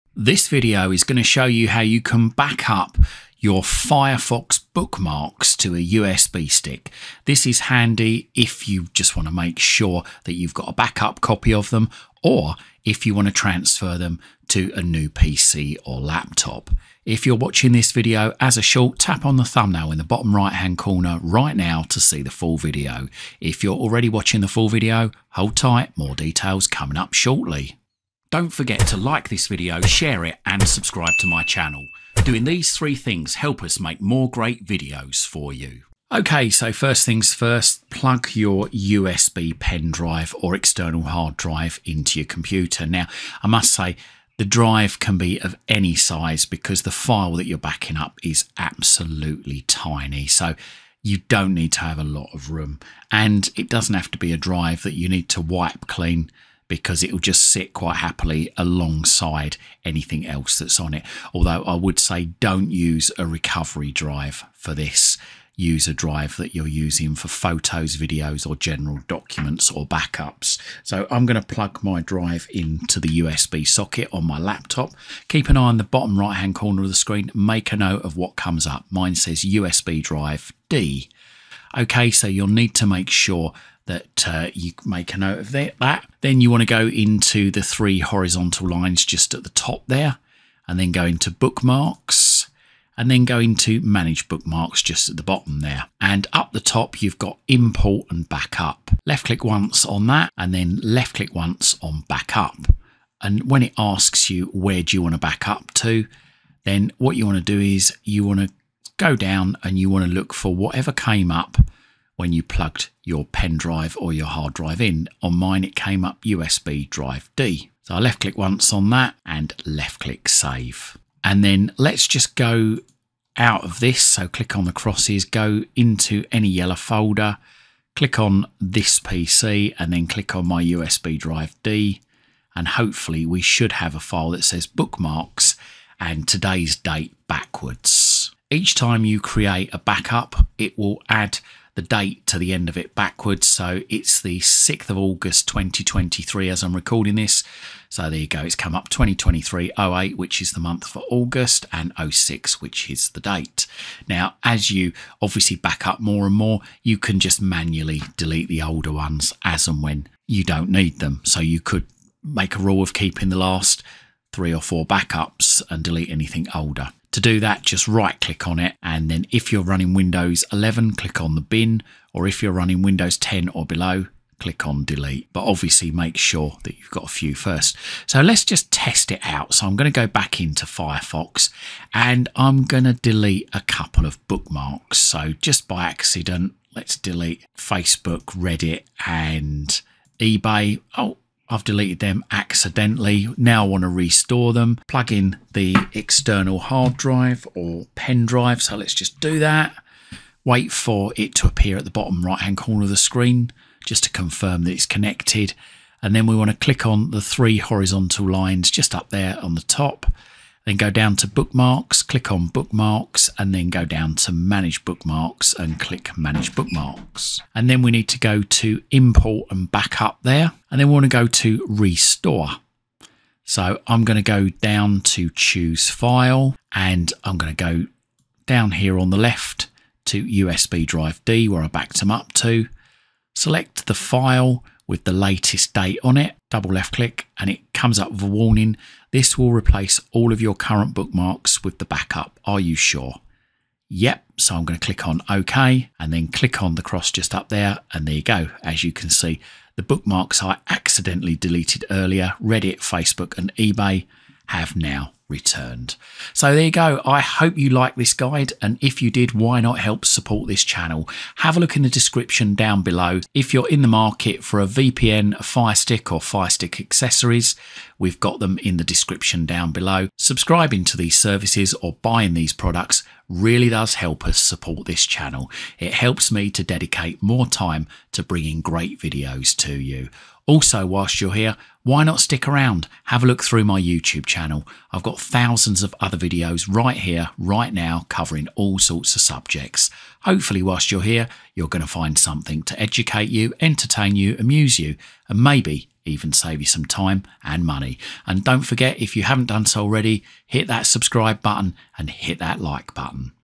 How To Save Firefox Bookmarks to a USB Stick
Shows you how to backup Firefox bookmarks to a USB Stick.